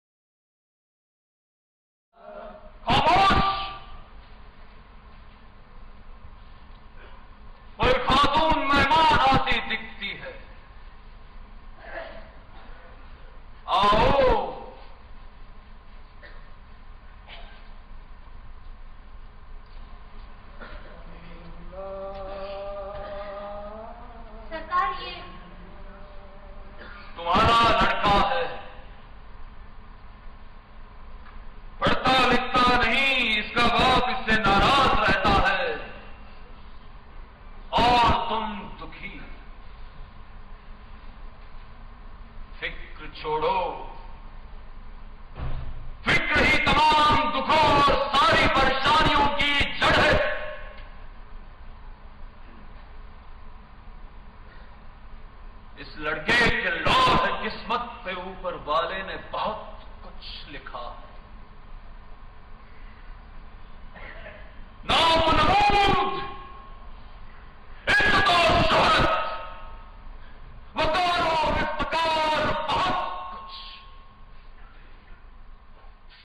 AMOL DESHMUKH ACTING REEL - PART THREE
Amol Deshmukh has graduated from the Film and Television Institute of India. Prior to that, he has had a vast amount of experience in theatre, film and television. These are just a few glimpses of his work.